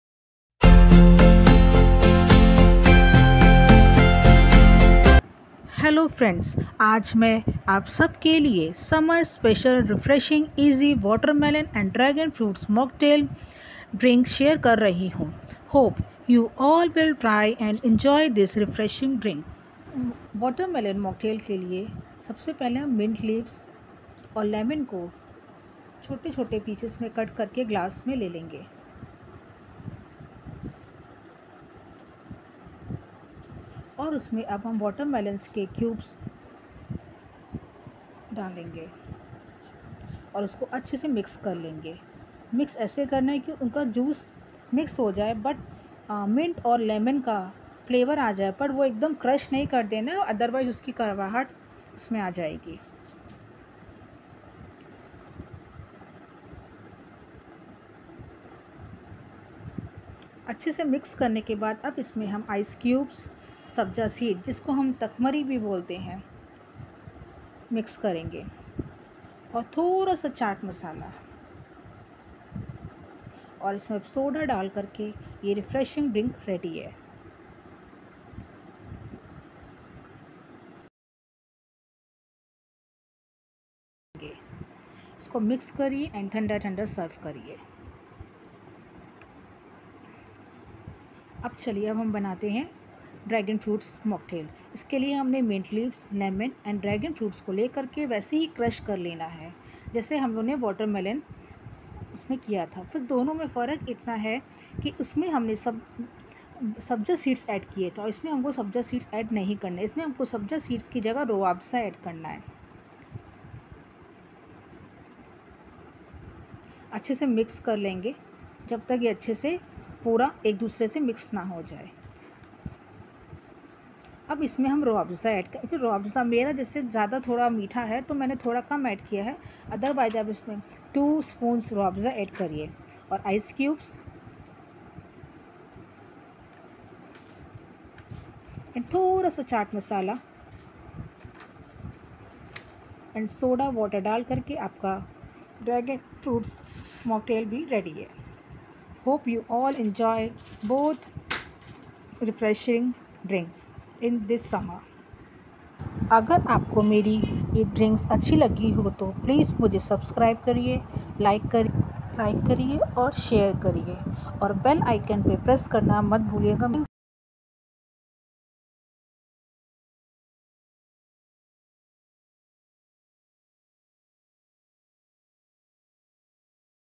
Tarbooj Sharbat | watermelon lemonade | watermelon juice | तरबूज का जूस | Dragon fruit juice
tips to remember*👇👇👇👇👇

👉when muddlling the mint leaves and lemon don't be too aggressive, as we want to extract the juice well, not the mint leaves to tear and shred  into pisces.

👉Make it less sour by adding little lemon.
👉Add sugar syrup if fruits are not so sweet.

👉 Receipe for Watermelon moktails

*Approx 15 fresh mint leaves
* Water melon cut in small pieces
*Two tablespoon Basil seeds ( soaked in water)
 *5 to 7 ice cubes
*chilled soda water
*chat masala
*sugar syrup if required*

👉 Receipe ---for dragon fruits mocktail
*Dragon fruit 1 bowl cut in small pieces
*Fresh mint leaves
* 5-7 Ice cubes
* Chilled soda water
* chat masala
👉👉👉👉👉 dragon fruit mocktail instead of basil seeds please add Rose syrup 2 to 3 spoon